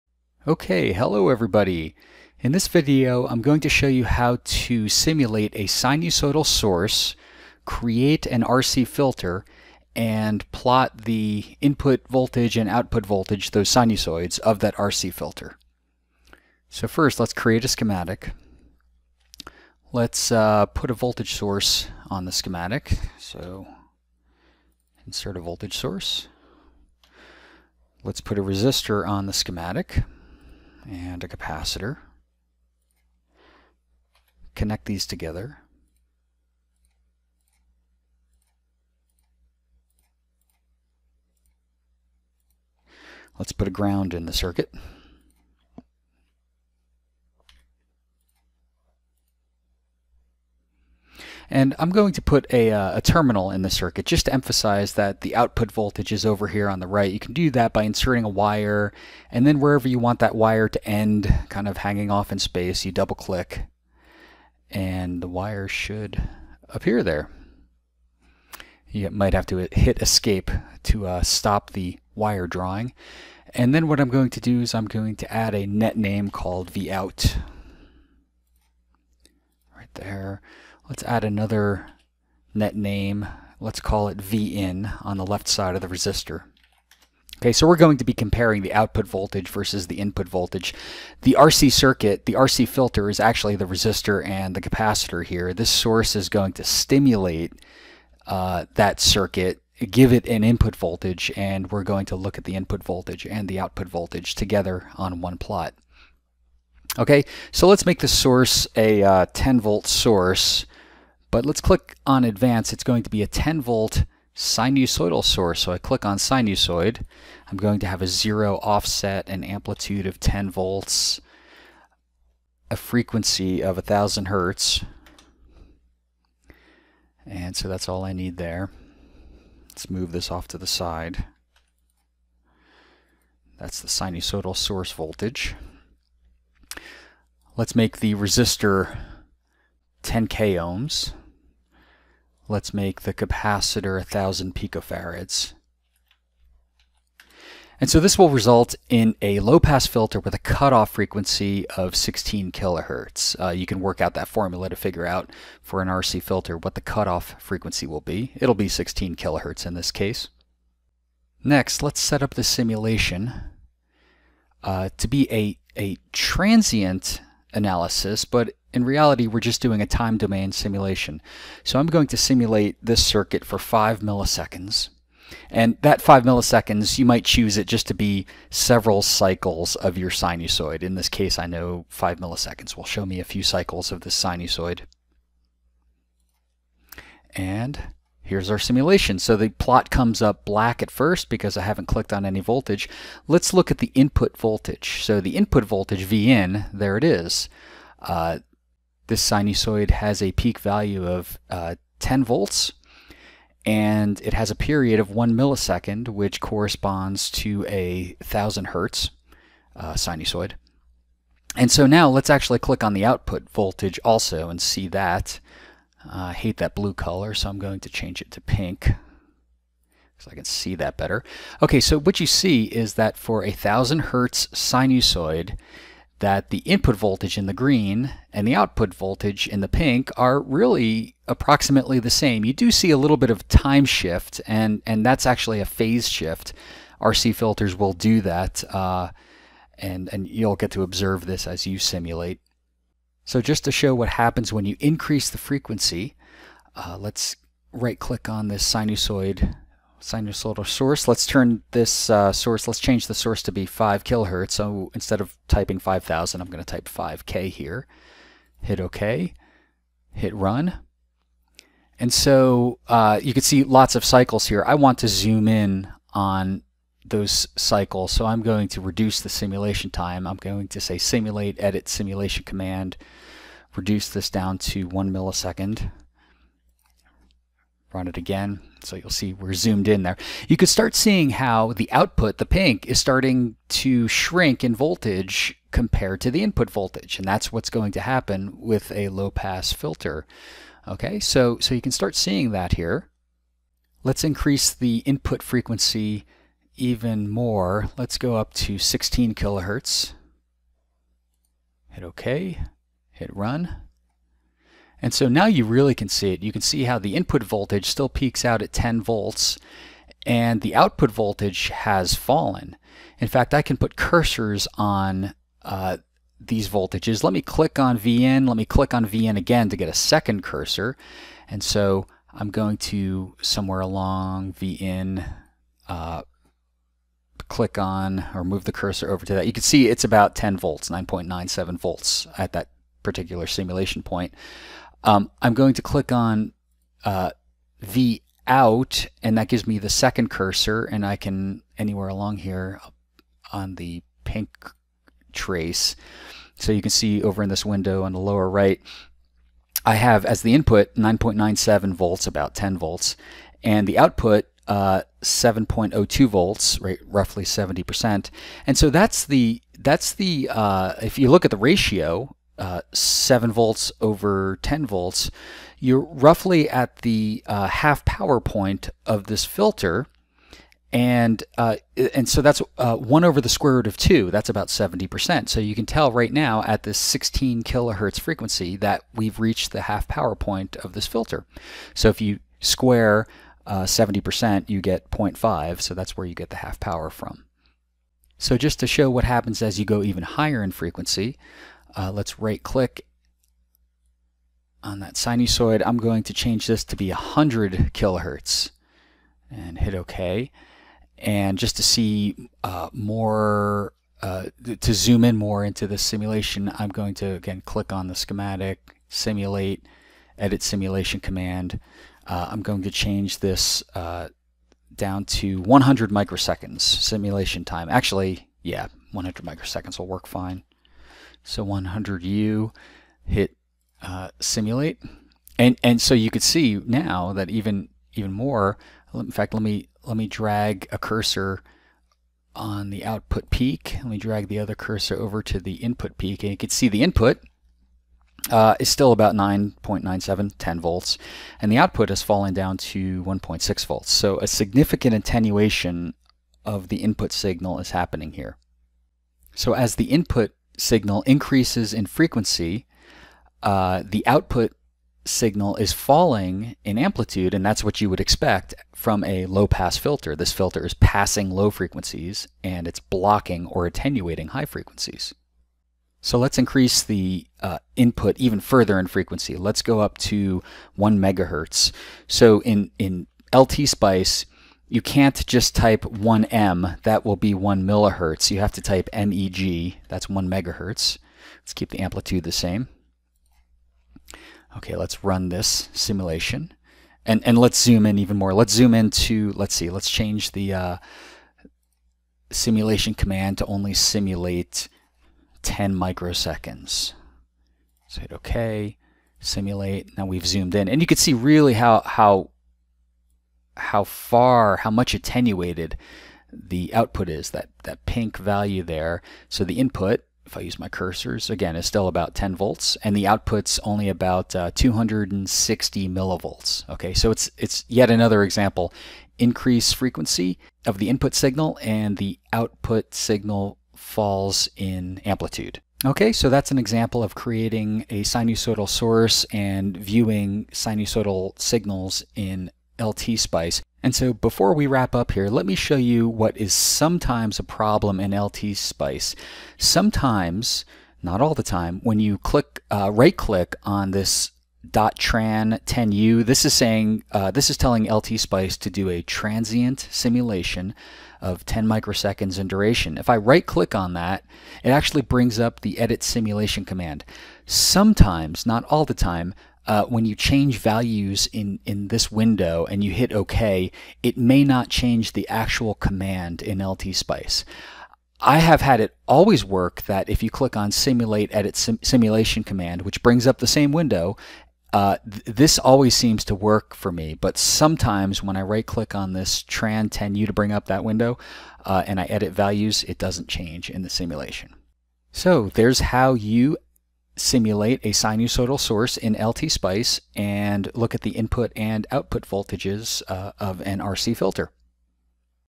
Simulating a Sinusoidal Voltages and an RC Filter in LTspice
Simulating a sinusoidal voltage source and measuring sinusoidal voltages at the input and output of an RC filter.